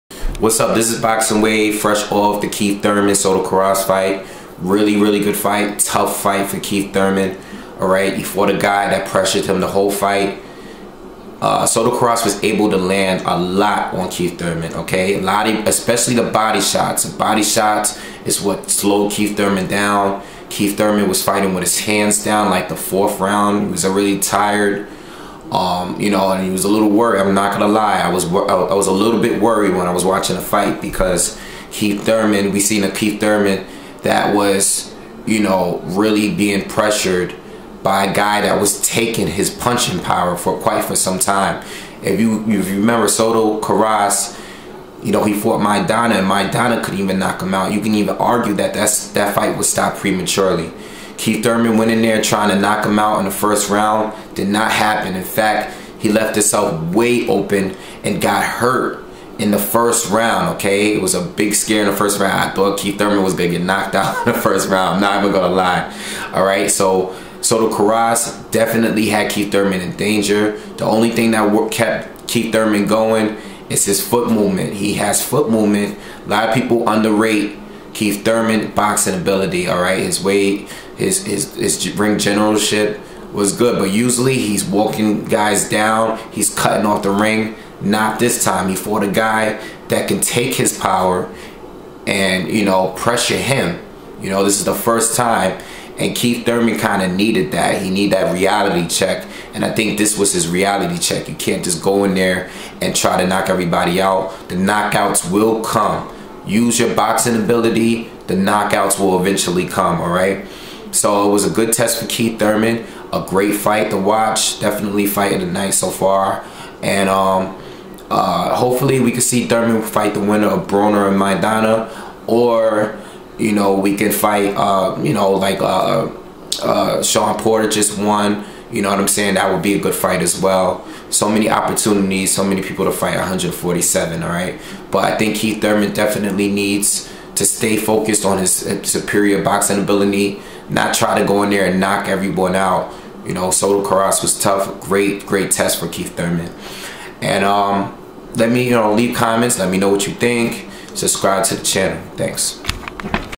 Keith Thurman knocks out Soto Karass!!!
Keith Thurman KO's Jesus Soto Karass post fight review!